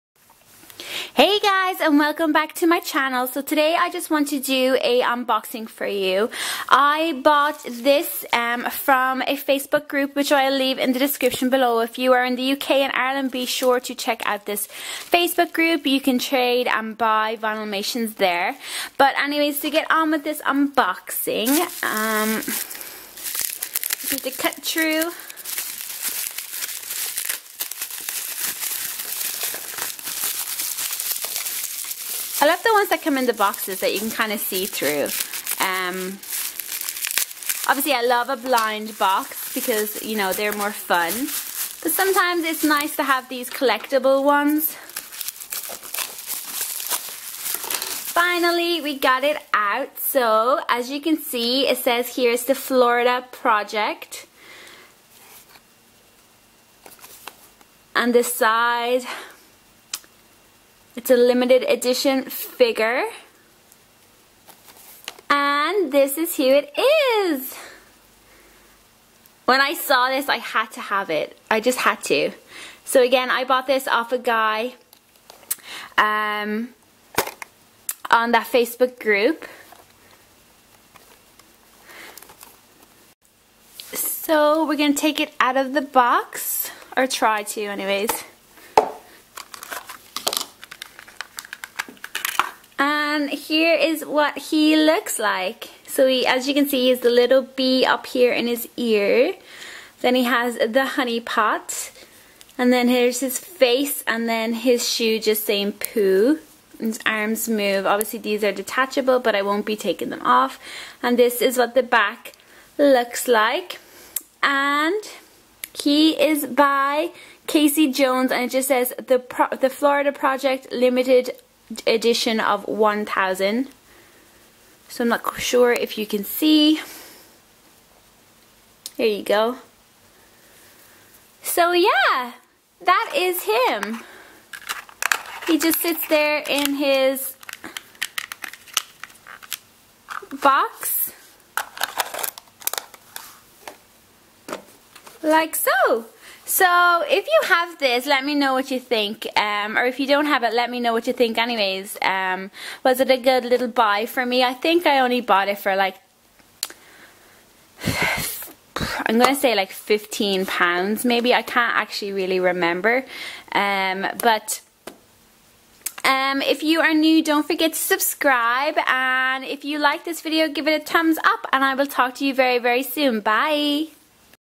Vinylmation - Florida Project 2011 - Disney Vinylmation Opening - Episode #10
I bought this very cool Florida Project 2011 Vinylmation from the UK group listed below. I really love the design of this vinylmation. Hope you can join me again next time for more unboxing of vinylmations.